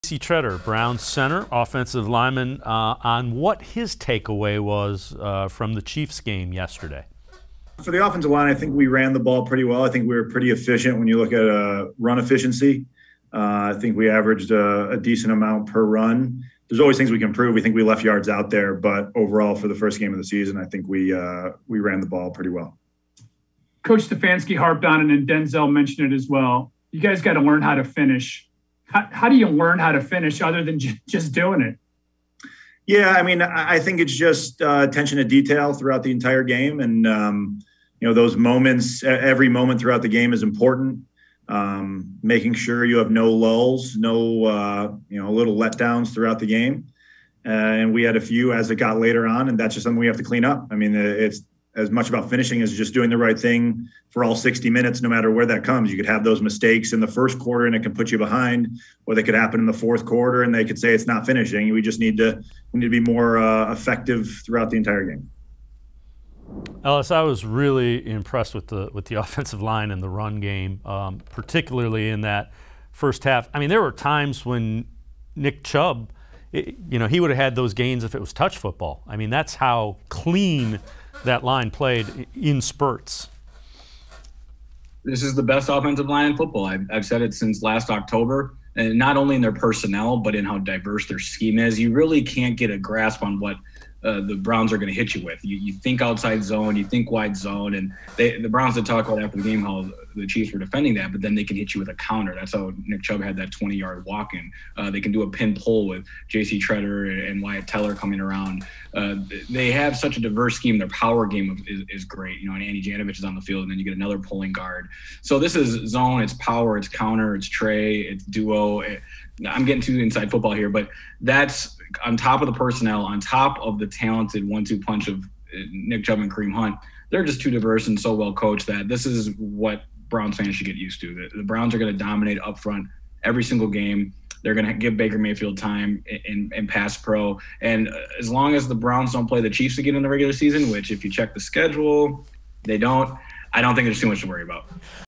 JC Tretter's Takeaways on the Offensive Line & the Browns as a Whole Week 1 - Sports 4 CLE, 9/13/21
Dave Bacon and Ellis L. Williams respond to what Browns center J.C. Tretter had to say on the Browns performance Sunday against the Chiefs.